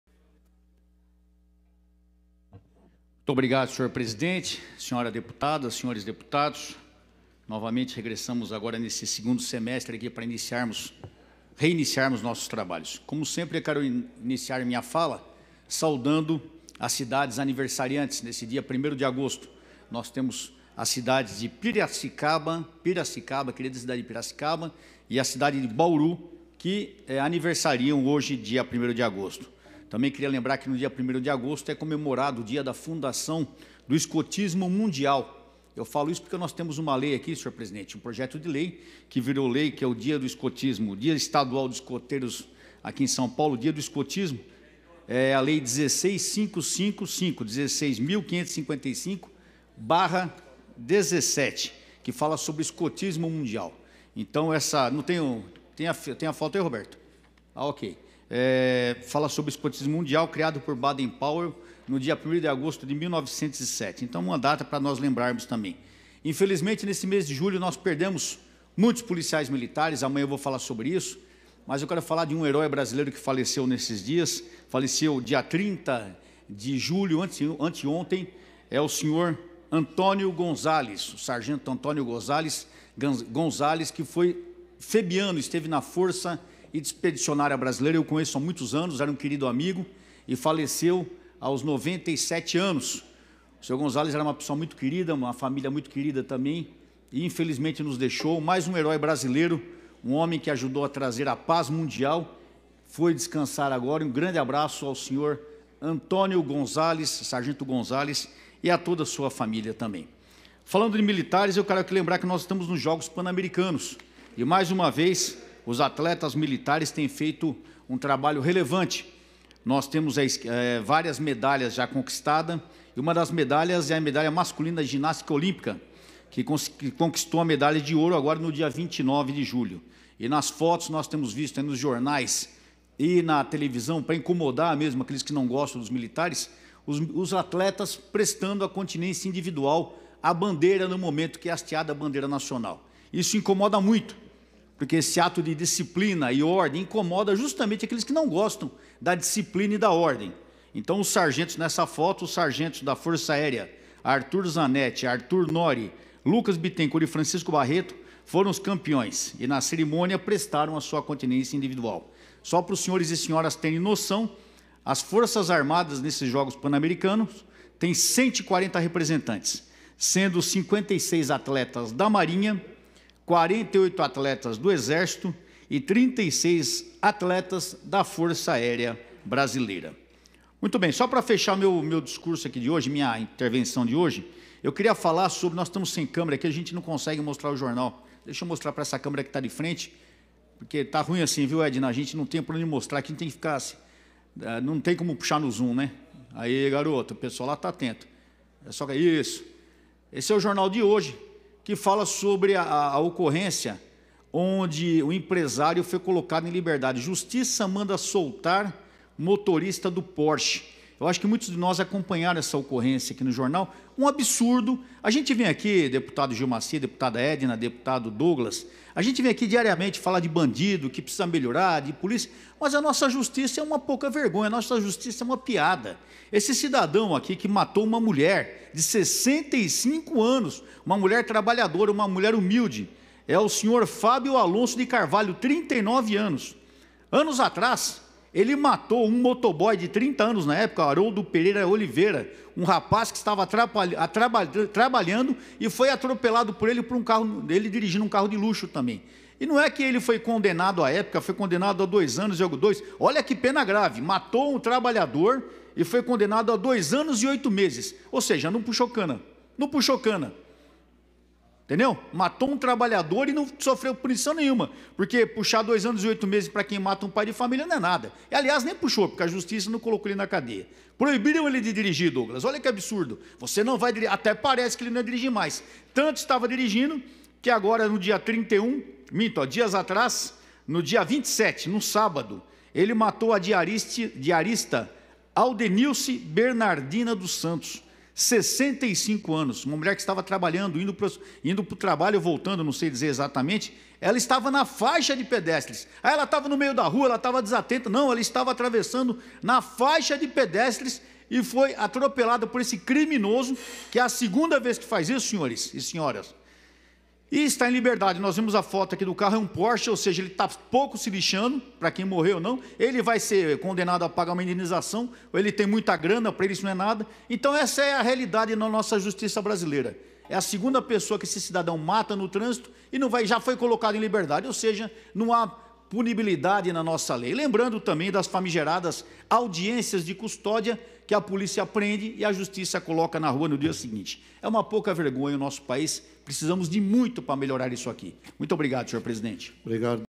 Coronel Telhada em plenário 01/08/19 - Pronunciamento na ALESP
Parabeniza as cidades de Piracicaba e Bauru pelo seu aniversário. Lembra que hoje é comemorado o Dia Mundial do Escotismo. Informa o falecimento do sargento Antônio Gonzalez, que fez parte da Força Expedicionária Brasileira. Elogia o desempenho de integrantes das Forças Armadas nos Jogos Pan-Americanos. Comenta o fato da Justiça brasileira tratar criminosos com leniência.